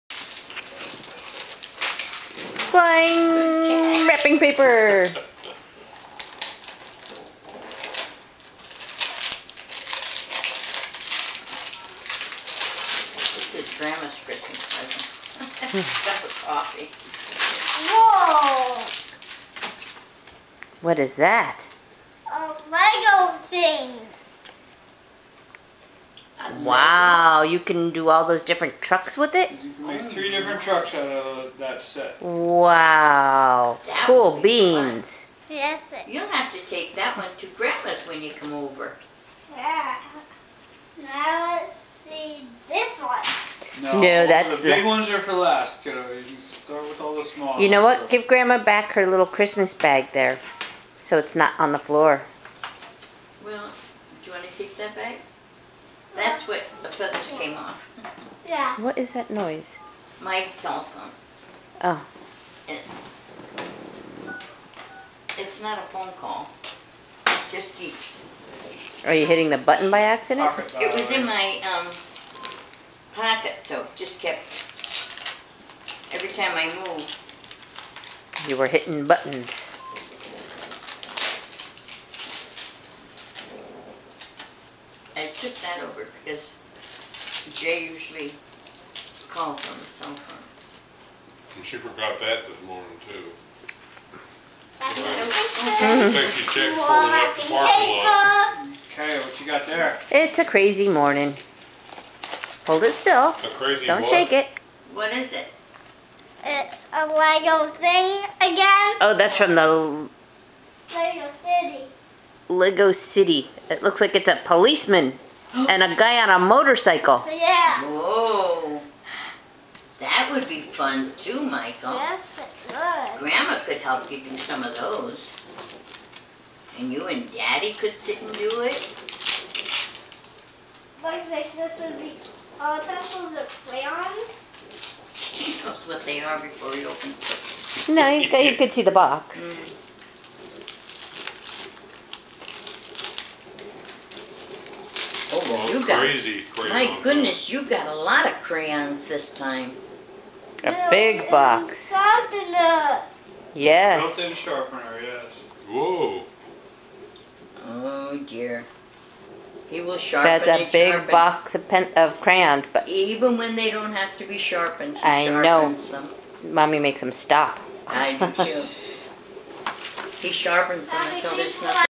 2014 12 24 Legos & Crayons
Michael opens more Christmas presents